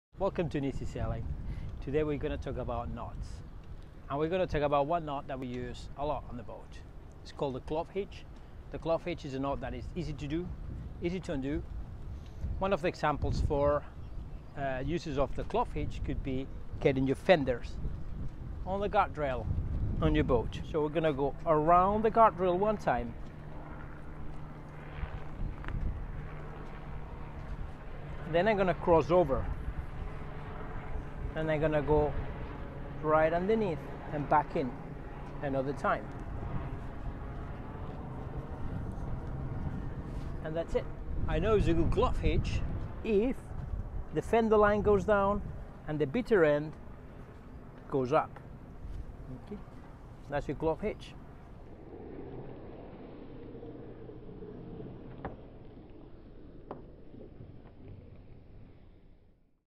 How To Tie A Clove Hitch Sailing Knot
NISI Sailing’s Captain Alex shows how to tie a Clove Hitch Sailing Knot - in under one minute. Come sailing with us in the Greek Islands aboard our sailing yacht, NISI!